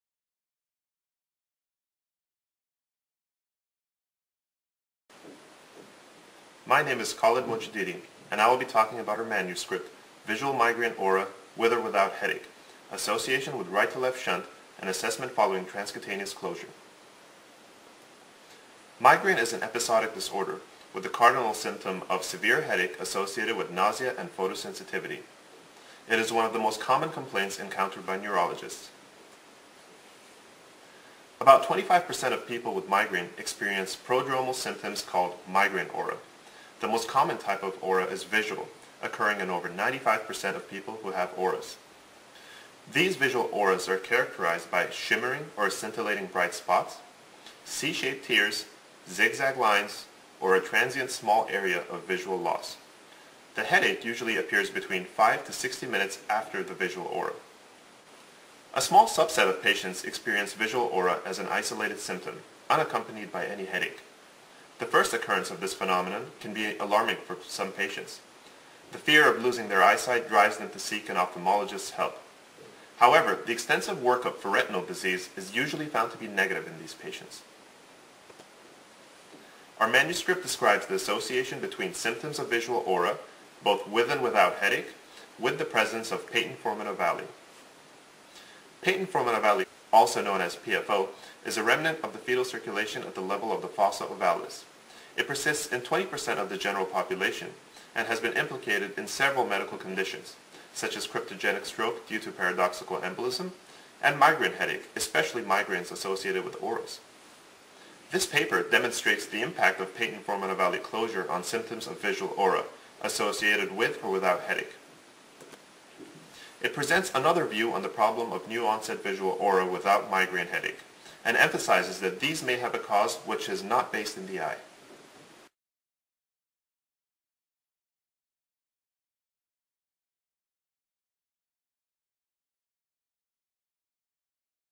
Visual migraine aura with or without headache - Video abstract: 30999
Video abstract of original research "Visual migraine aura with or without headache: association with right to left shunt and assessment following transcutaneous closure" published in the open access journal Clinical Ophthalmology by M Khalid Mojadidi, Hamidreza Khessali, Rubine Gevorgyan, et al.

Conclusion: There is an increased prevalence of PFO among patients with migraine aura without headache. The closure of PFO correlates with improvement of the visual aura, suggesting a causative association between the presence of PFO and both visual aura and migraine headaches. Ophthalmologists should be aware of the association of right to left shunts with visual aura.

Read this original research and sign up to receive Clinical Ophthalmology